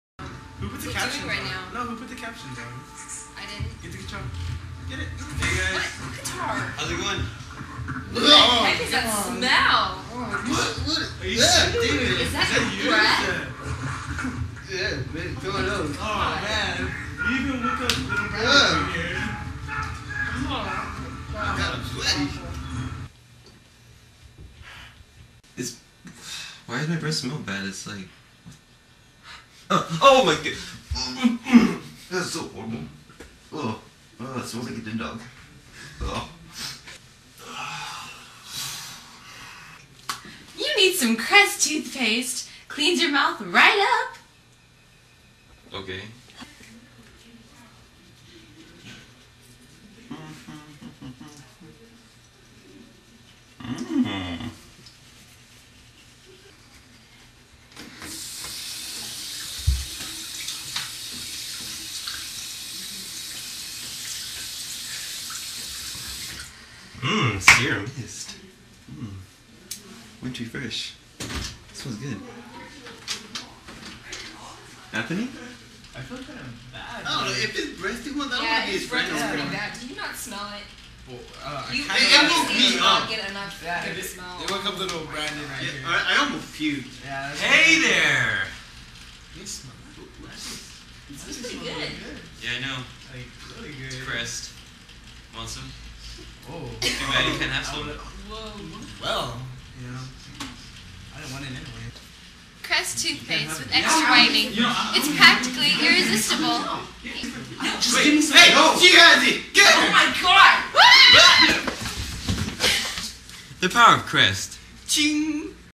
Crest Toothpaste
A commercial from my Eng 11 CP video project :D (yeah I know, the quality isn't that great) [June 2008]